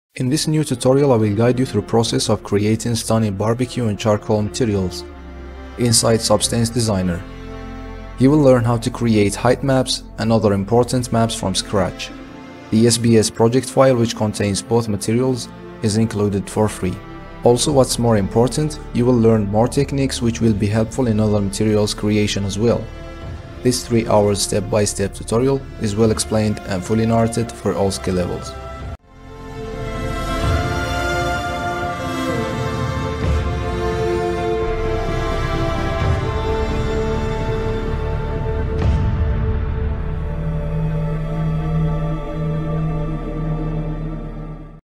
BBQ & Charcoal Materials Creation Tutorial on Substance Designer
Here's the tutorial I promised you about I hope you all find it useful!

In this new tutorial I will guide you through process of creating stunning BBQ & Charcoal materials inside Substance Designer.

You will learn how to create height maps & other important maps from scratch.

The SBS project file which contains both materials is included for free.

Also what's more important, you'll learn more techniques which will be helpful in other materials creation as well.

 This 3 hours step-by-step tutorial is well explained and fully narrated for all skill levels.